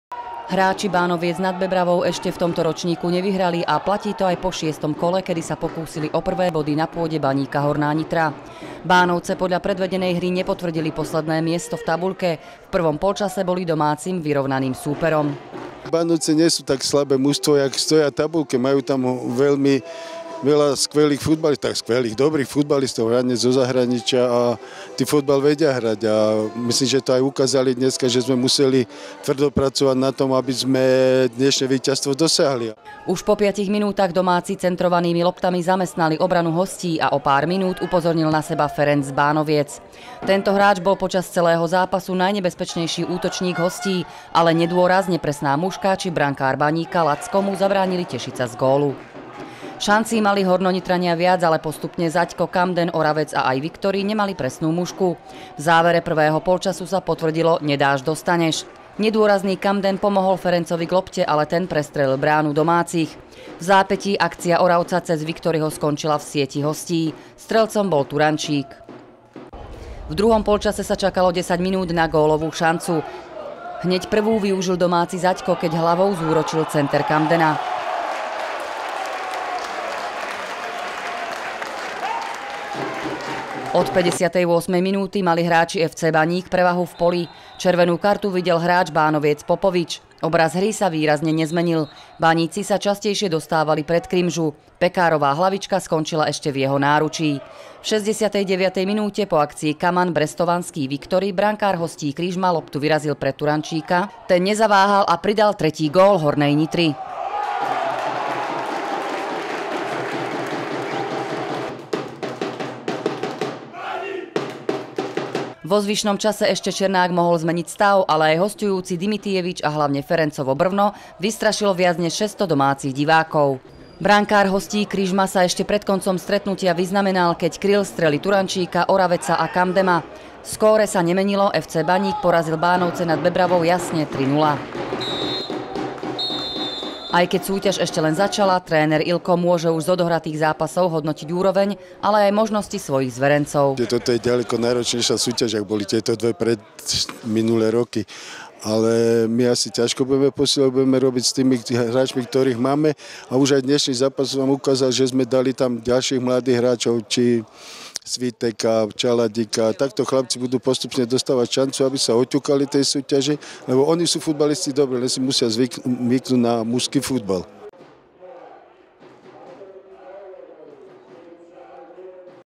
FC Baník už na štvrtom mieste
Hráči Bánoviec nad Bebravou ešte v tomto ročníku nevyhrali a  platí to aj po šiestom kole, kedy sa pokúsili o prvé body na pôde Baníka Horná Nitra. Bánovce podľa predvedenej hry nepotvrdili posledné miesto v tabuľke. V prvom polčase boli domácim vyrovnaným súperom.